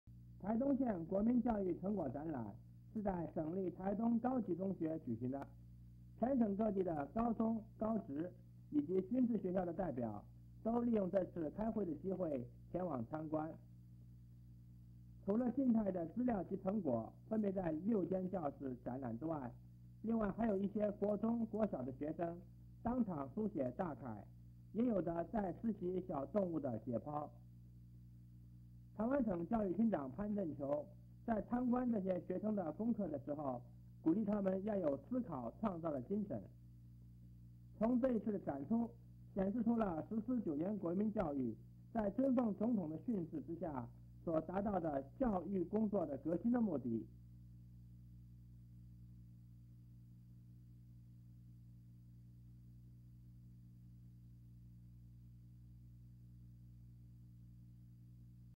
台東縣國中小國民教育展覽會
(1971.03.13)台東縣國民教育成果展覽在省立台東高級中學舉行，全省各地的高中高職代表亦前往參觀，除在六間教室展覽外，在其他教室有學生當場書寫大楷.教育廳長潘振球亦到場參觀，並勉勵學生要有思想創新的精神.